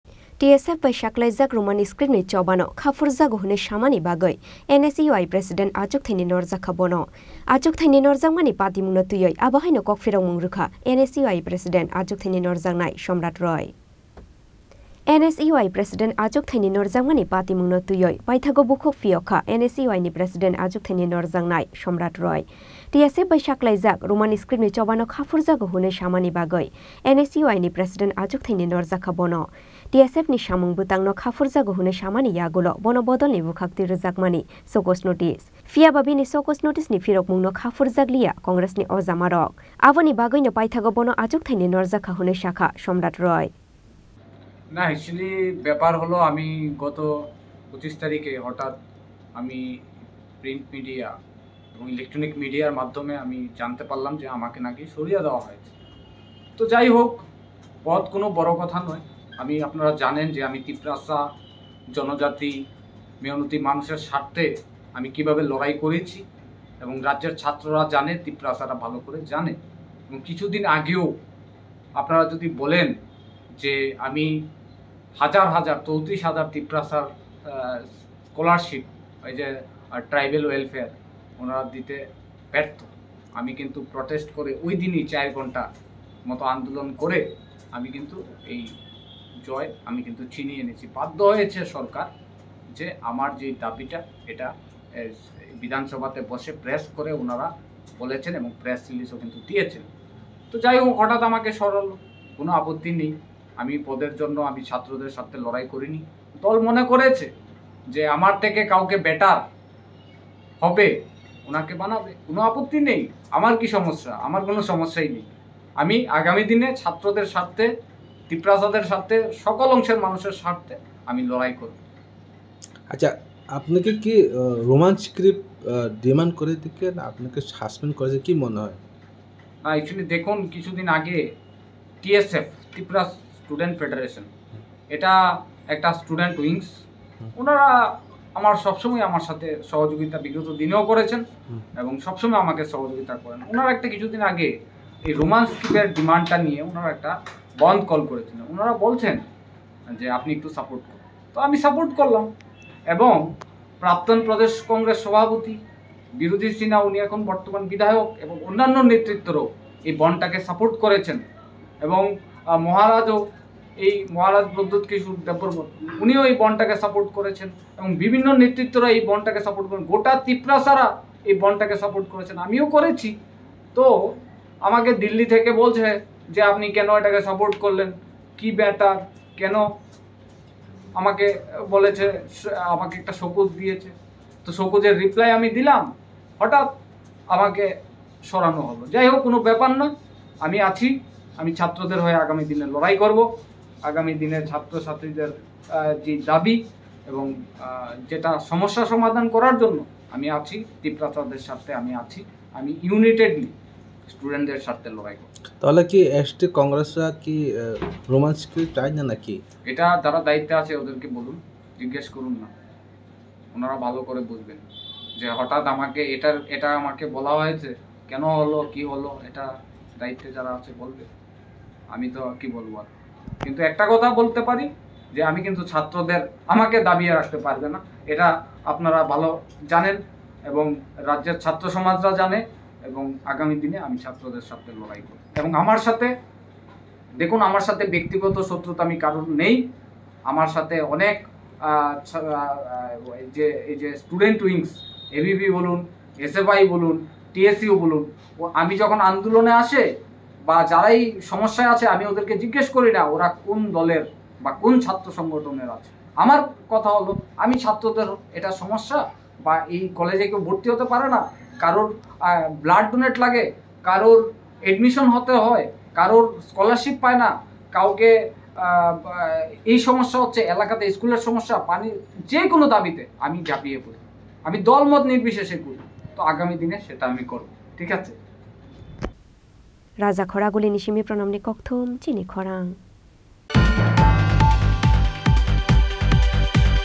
Roman Script no twiwi TSF ni Bandno Khaphurjagwi Samrat Roy no NSUI President Achukthai ni Norjakha
🟢 TSF bai Saklaijak Roman Script ni Choba no Khaphurjago honwi Samani bagwi, NSUI President Achukthai ni Norjakha Bono. Achukthai ni Norjakmani Patimungno twiwi Abo haino Kok Phirokmung Rwkha NSUI President Achukthai ni Norjaknai Samrat Roy.